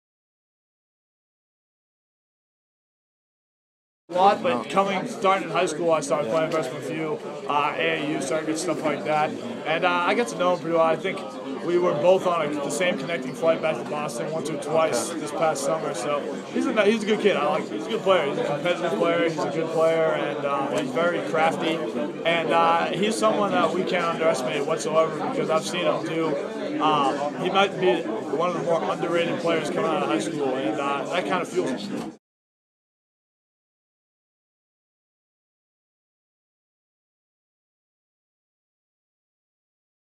Notre Dame's Pat Cannaughton talks Georges Niang
Notre Dame's Pat Cannaughton discusses fellow Boston-area product Georges Niang of Iowa State prior to the team's meeting up in the Round of 64 in the 2013 NCAA Tournament.